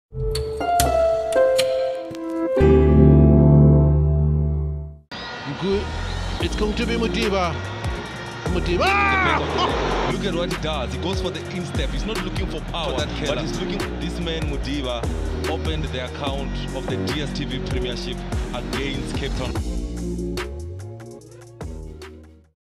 Lloyd Junior Kazapua sturning save vs Sundowns
from the match between Chippa United and Mamelodi Sundowns